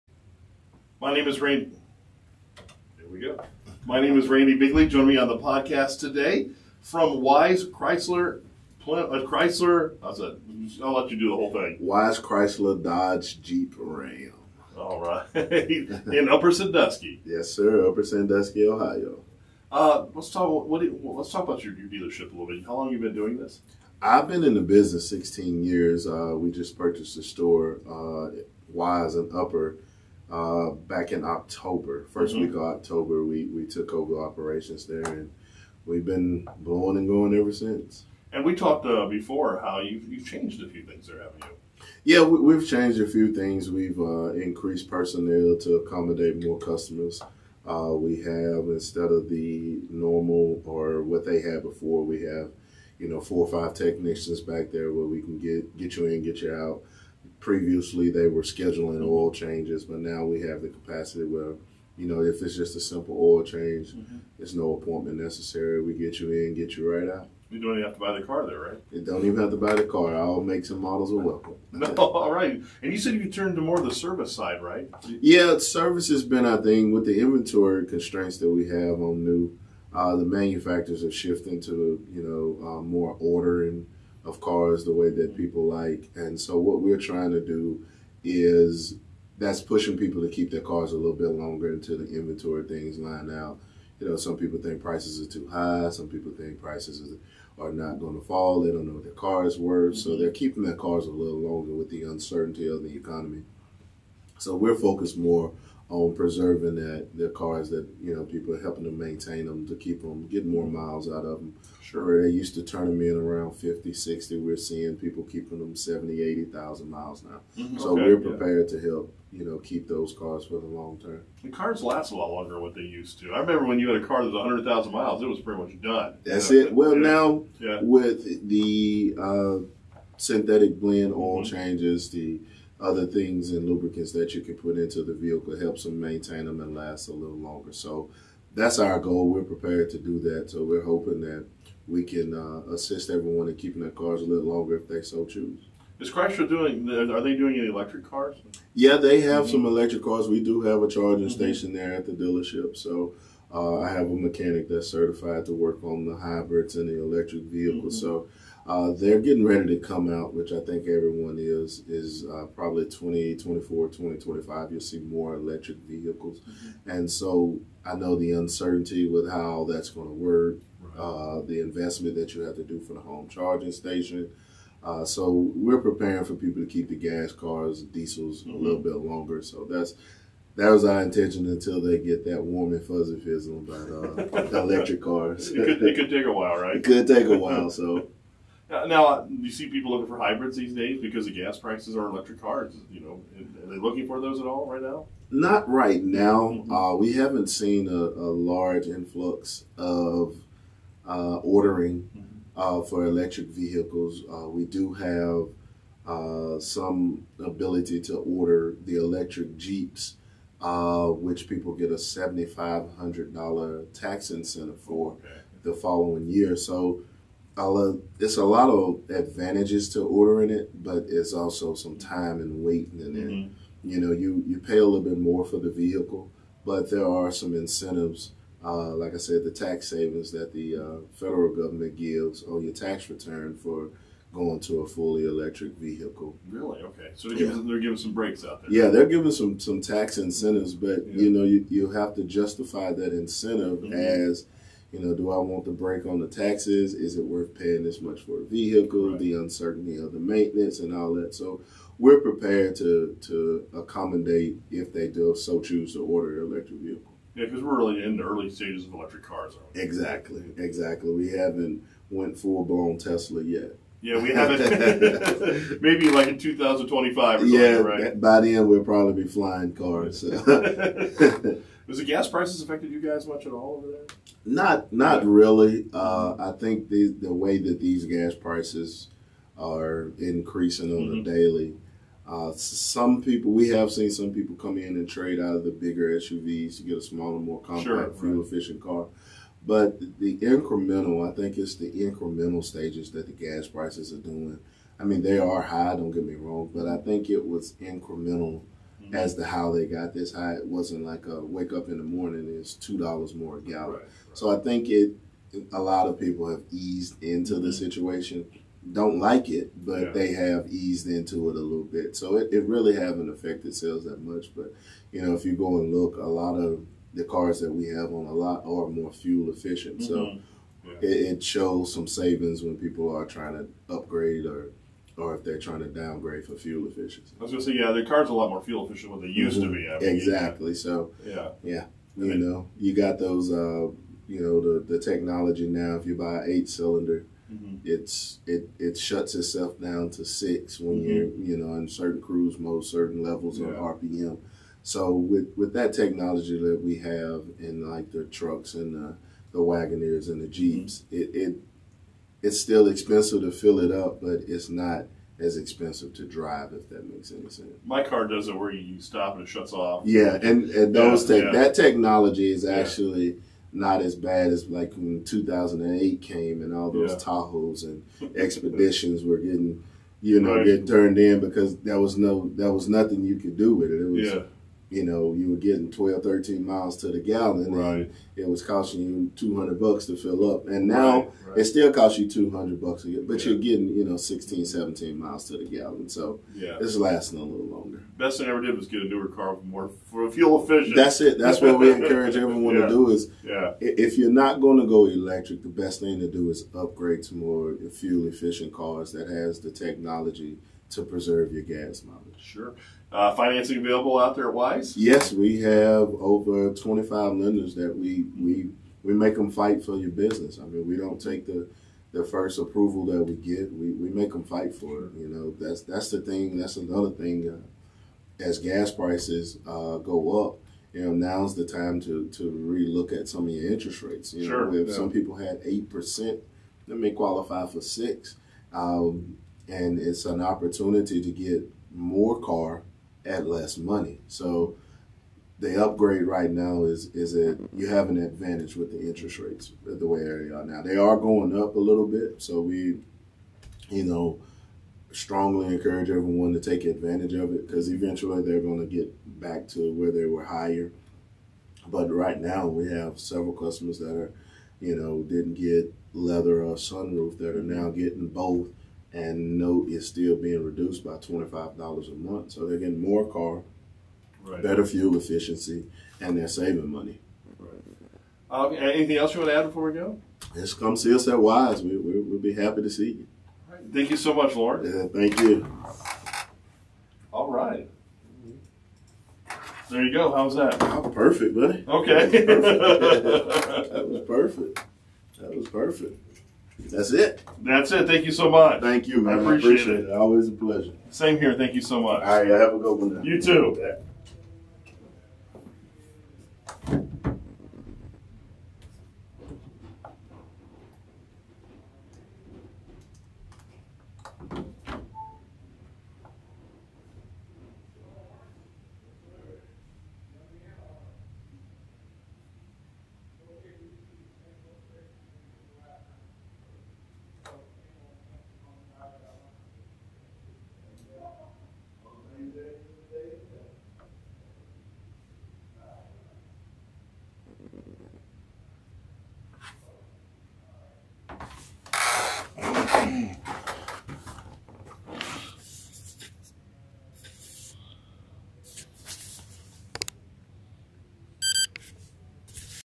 Community Spotlight Podcast: Wise Chrysler Dodge Jeep RAM
Lawrence Vaughn from Wise Chrysler Dodge Jeep RAM in Upper Sandusky talks with Randy about the inventory and services available at Wise CPDJ.